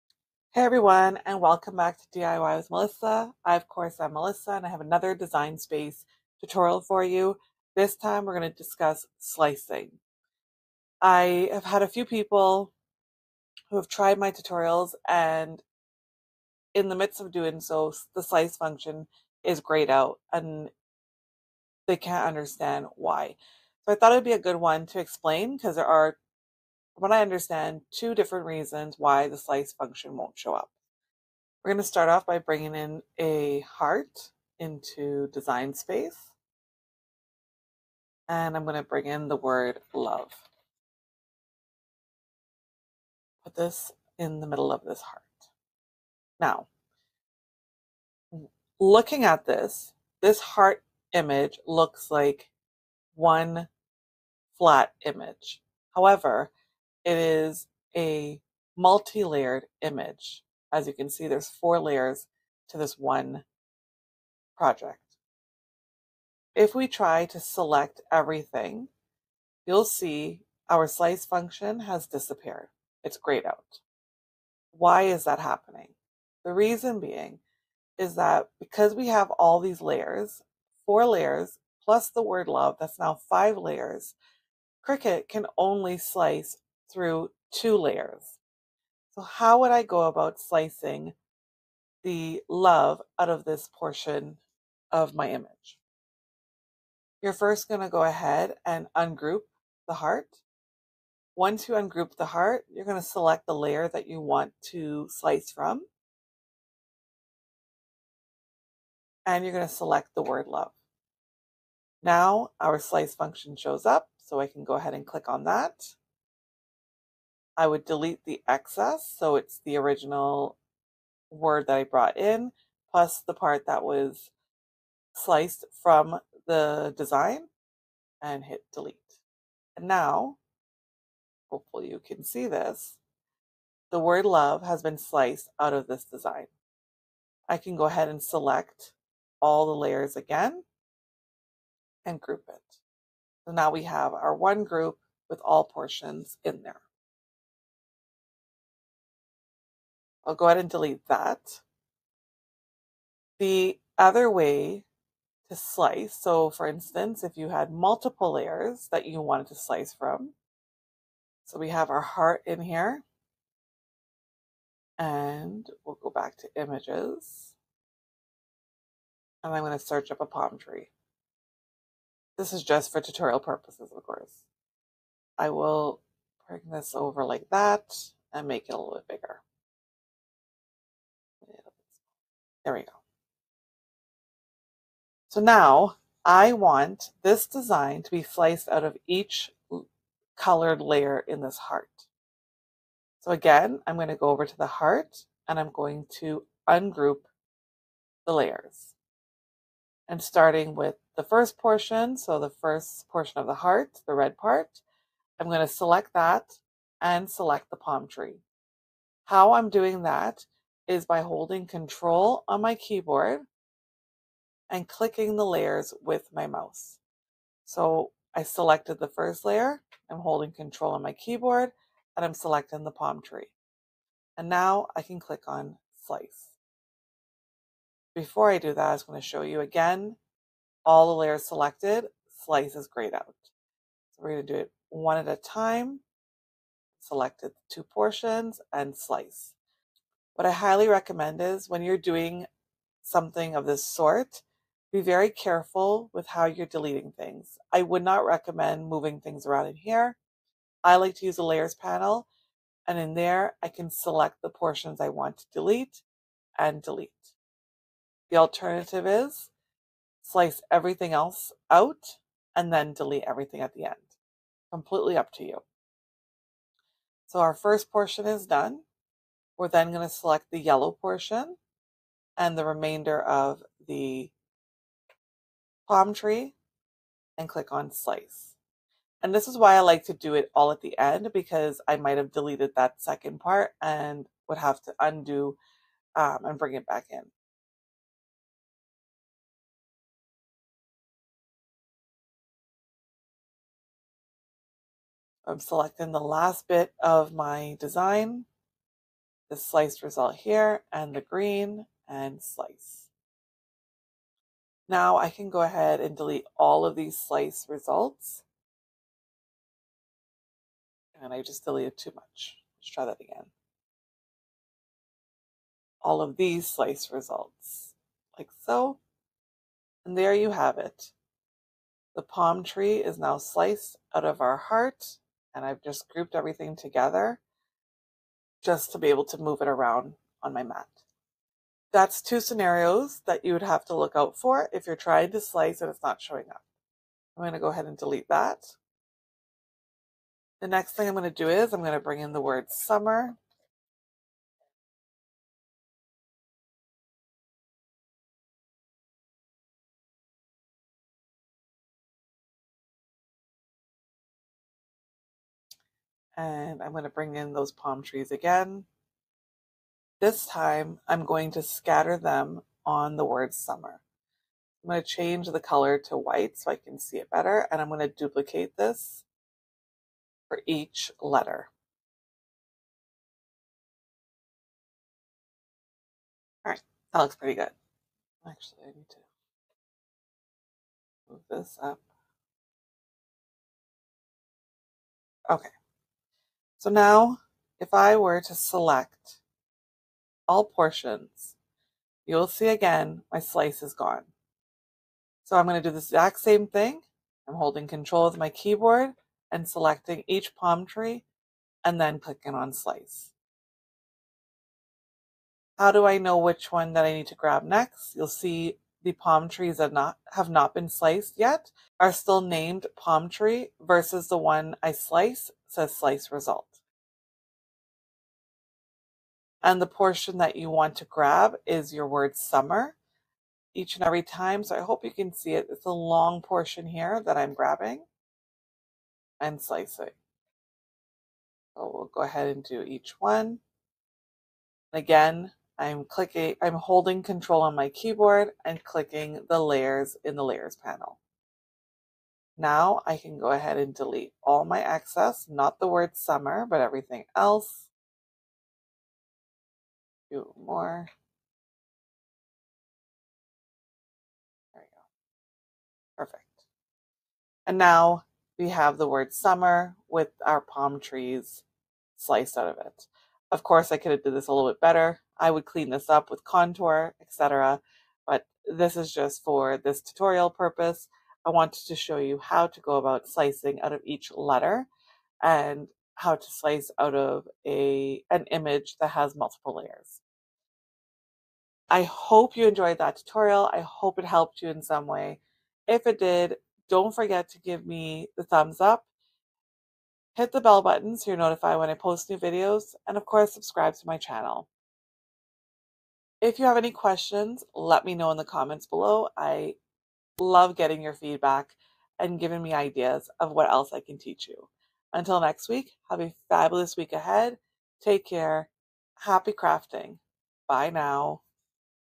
Why can't I slice in Cricut Design Space?!? Is your slice function greyed out? Find out why now!
Why Can’t I Slice in Cricut Design Space? 3 Common Problems Solved

Are you getting frustrated because the Slice tool in Cricut Design Space isn’t working? You’re not alone! In this video, I walk you through the 3 most common reasons why Slice won’t work—and how to fix them fast so you can get back to crafting confidently.
Whether you're designing custom stickers, decals, t-shirts, or paper crafts, understanding how to use the Slice feature correctly in Cricut Design Space is essential. I’ll show you exactly what to check for when the Slice button is greyed out or won’t activate, including layer selection tips, grouping issues, and shape compatibility.

This tutorial is perfect for Cricut beginners, DIYers, and crafters looking to master their design skills and troubleshoot common Cricut problems.

🛠 Great for Cricut Explore Air, Cricut Maker, and Cricut Joy users.